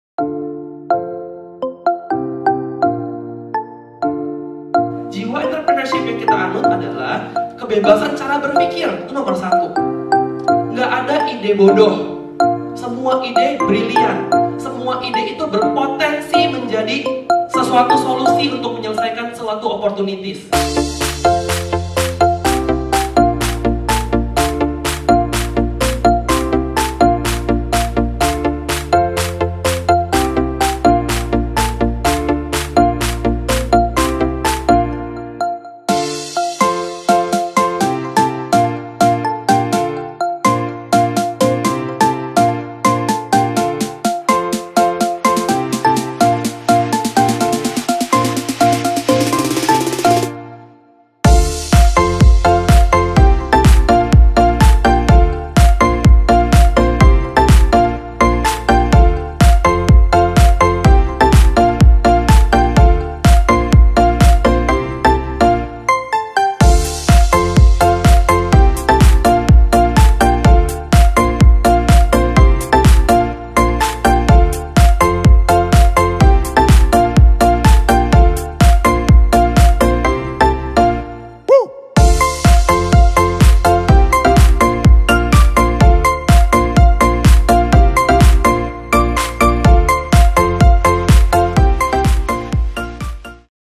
Campus Visit SMK Santa Maria
Keseruan dari salah satu campus visit bersama SMK Santa Maria mengunjungi Bisnis Perhotelan.

Kalian mau juga campus visit ke kampus kami? Bisa!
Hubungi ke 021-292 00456
Lihat juga acara campus visit lain di Instagram @podomorouniversity